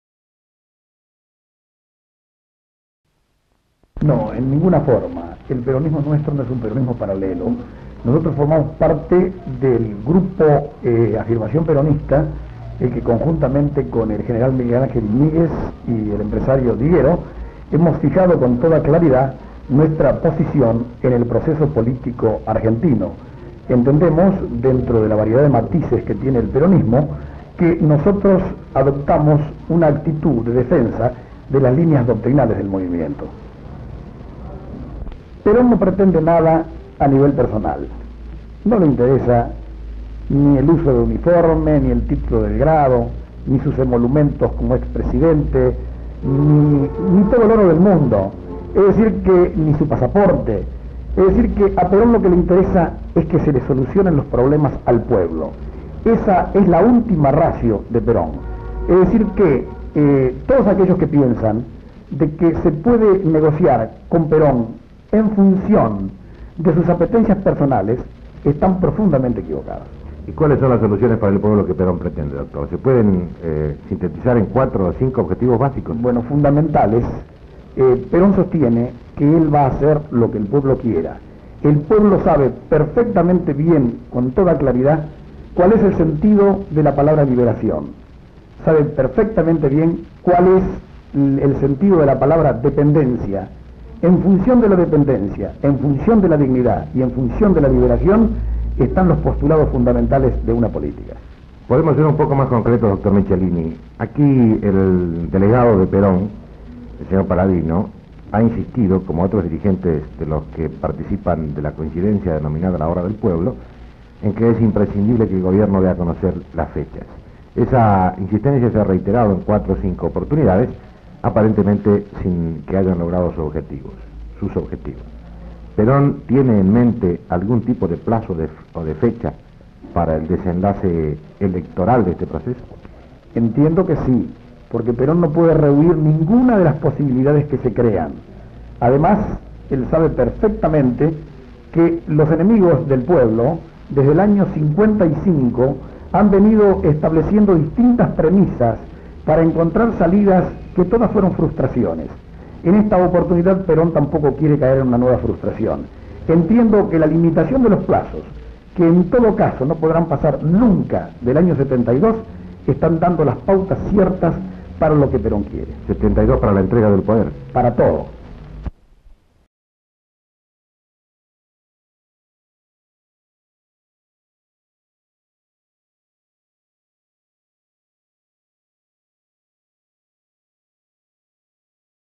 Pedro Michelini - El Peronismo nuestro no es un Peronismo paralelo 1971
Bs. As.: Reportaje al Dr. Pedro Michelini donde expresa: -No, en ninguna forma. El Peronismo nuestro no es un Peronismo paralelo, nosotros formamos parte del Grupo de Afirmación Peronista en que conjuntamente con el general Miguel Ángel Iñiguez y el empresario Diero hemos fijado con toda claridad nuestra posición en el proceso político argentino, en donde vemos dentro de la variedad de matices que tiene el Peronismo, que nosotros adoptamos una actitud de defensa de las líneas doctrinales del movimiento. -Peron no pretende nada a nivel personal, no le interesa ni el uso del uniforme, ni el título de grado ni sus emolumentos como ex presidente, ni todo el oro del mundo, ni su pasaporte, es decir, que a Peron lo que le interesa es que se le solucionen los problemas al pueblo. Esa es la última raseo de Peron, es decir, que todos aquellos que piensan de que se puede negociar con Perón en función de sus apetencias personales están profundamente equivocados. Luego el periodista pregunta: -¿Y cuáles son las soluciones para el pueblo que usted no pretende doctor, se pueden sintetizar en 4 o 5 objetivos básicos? -¿Podemos ser un poco más concretos Dr. Michelini, aquí el delegado de Peron, el señor Paladino ha insistido como otros dirigentes de los que participan de la coincidencia denominada La Hora del Pueblo en que es imprescindible que el gobierno dé a conocer las fechas, esa insistencia se ha reiterado en 4 o 5 oportunidades aparentemente sin que haya logrado sus objetivos, Perón tiene en mente algún tipo de plazo o de fecha para el desenlace electoral de este proceso? -¿1972 será para la entrega del poder? (Sergio Villaroel)
Fecha: 9/6/1971
Duracion: 2 minutos 54 segundos
B-03973